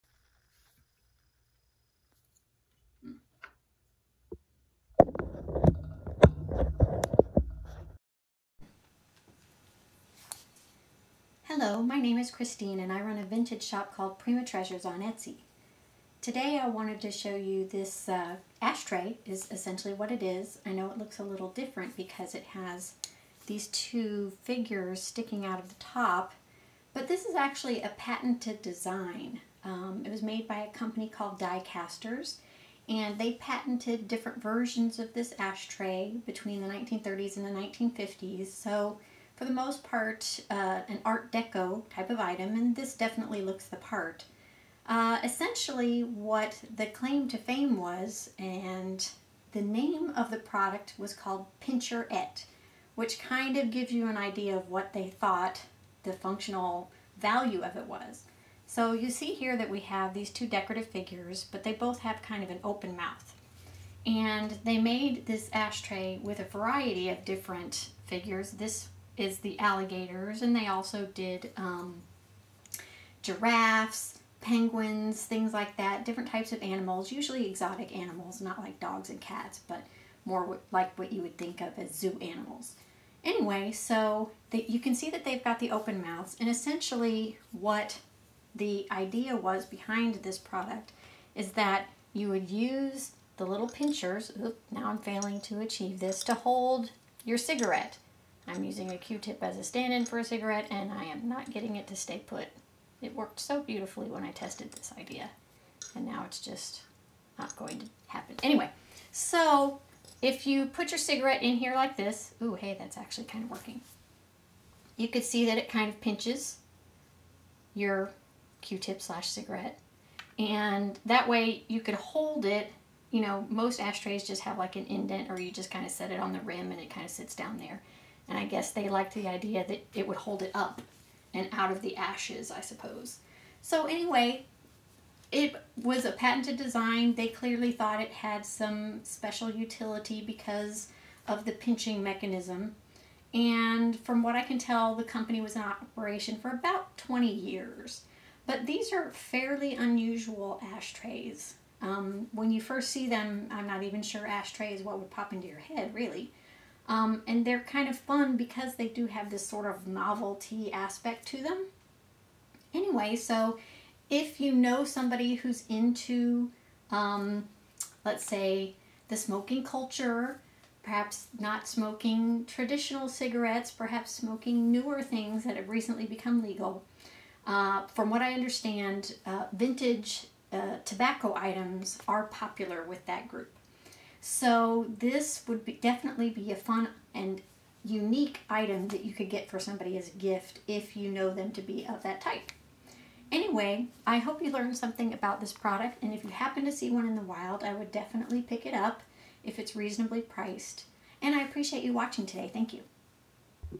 Art Deco "Pincherette" Ashtray
This week I'm sharing a figural novelty ashtray called a "Pincherette" that was made from the 1930s to the 1950s: